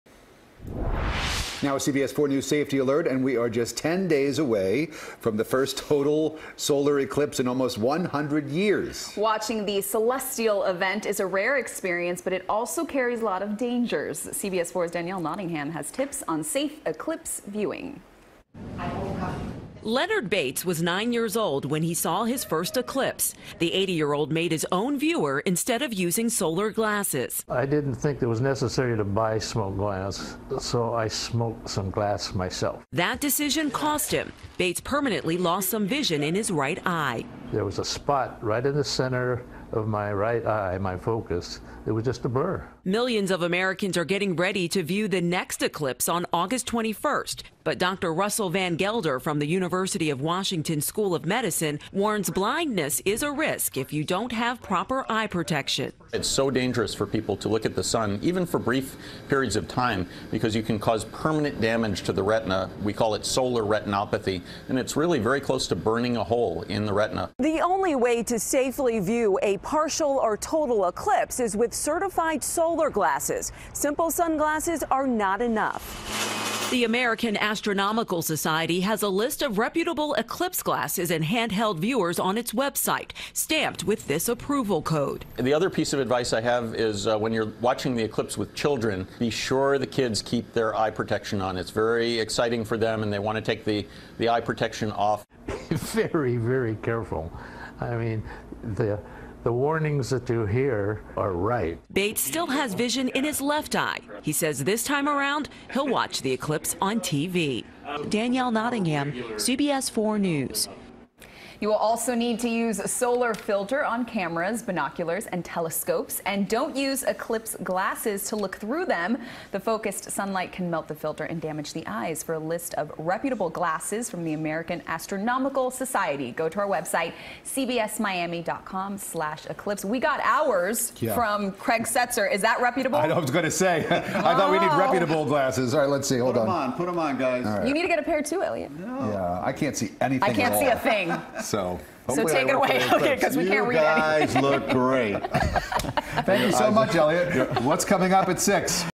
Doctors Stress Proper Eye Protection For Viewing Solar Eclipse
Danielle Nottingham reports.